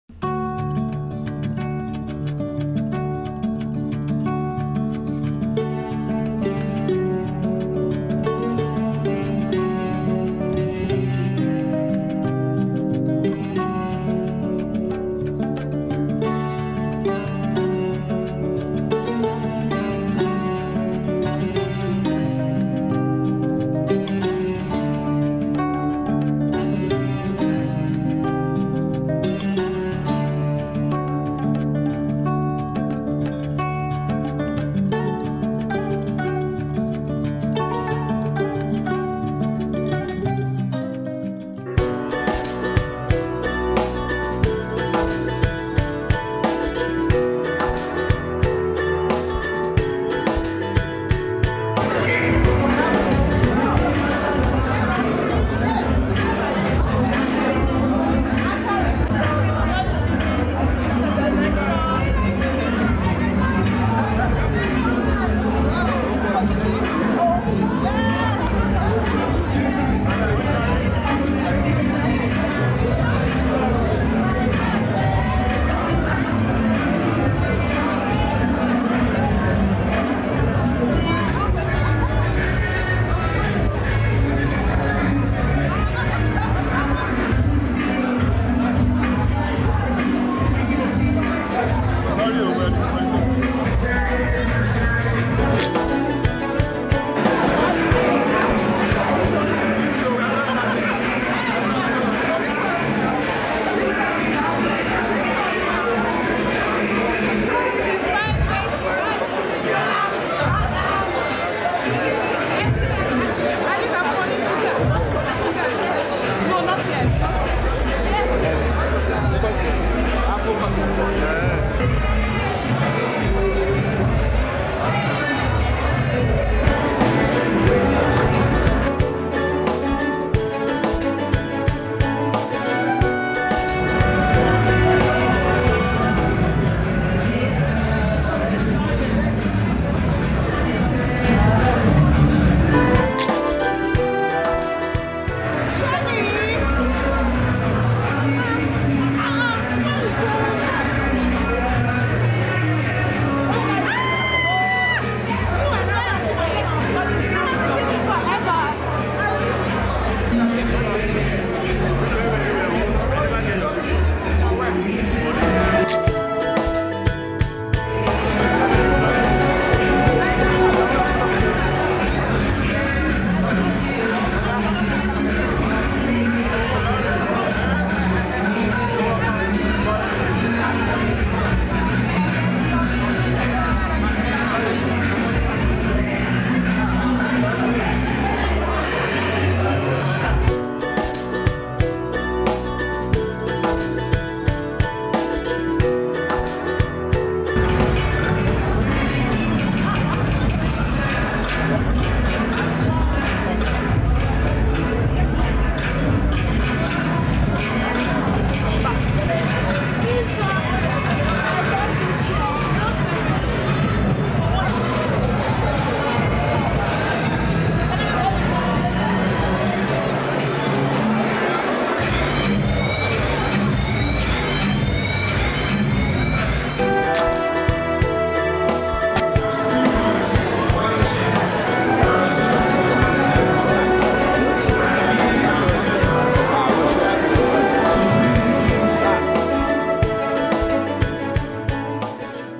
Funke Akindele Steps Out For "Sugar Rush" Movie Premiere
Hey Guys ! I’m Sorry it’s been a while I updated you all on My Activities. I’ve been busy working on my directorial debut movie “Your Excellency” , Yes guys, I Directed this movie and it’s showing now in all cinemas Nationwide, Go watch it !

 I had an amazing time  networking with people, Met my lovely friends , Uche Jombo, Kemi Adetiba , Toke Makinwa amongst others.

Thanks to my glam squad for adding extra sugar and spice to my look.. Rate My outfit over 10, How did I look ?